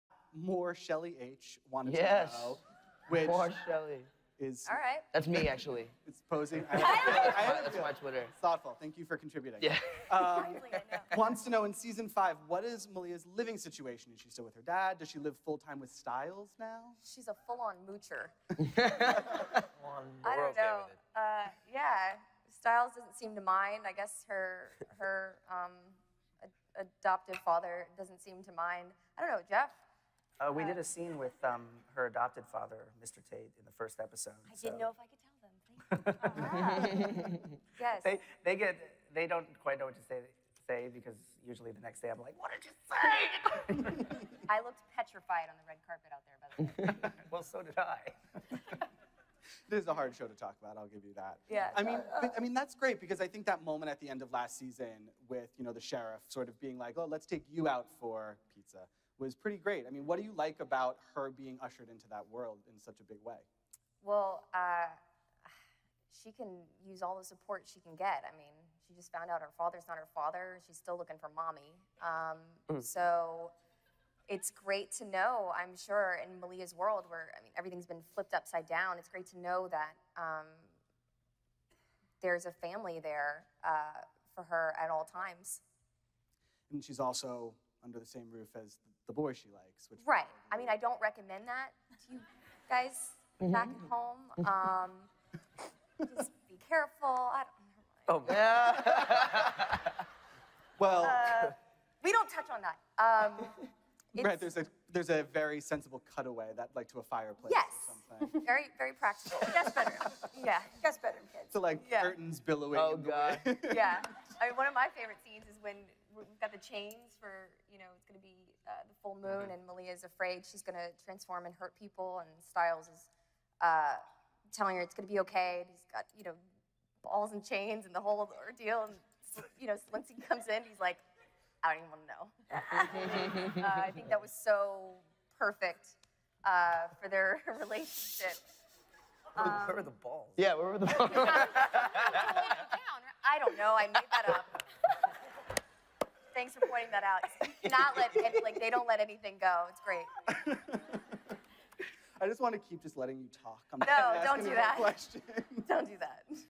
Teen Wolf cast talks about Malia in season 5 (Paleyfest)
Shelley Hennig, Jeff Davis, and Dylan O'Brien talks about Malia Tate's living situation in season 5, her relationship between her and Stiles Stilinski, and some of her funny scenes in season 4.
Attending the 2015 Teen Wolf Paleyfest are Holland Roden, Shelley Hennig, Dylan O'Brien, Tyler Posey, Arden Cho, Dylan Sprayberry, Jeff Davis, and Crystal Reed.
The Paleyfest was held on March 11, 2015.